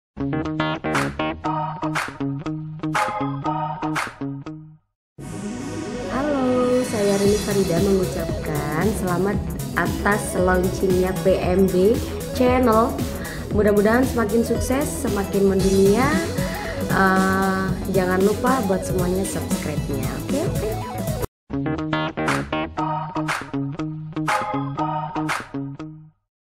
PMB_Channel - RENI FARIDA
Ucapan selamat dan sukses atas lounchingnya PMB Channel oleh Artis Top Jawa Timur RENI FARIDA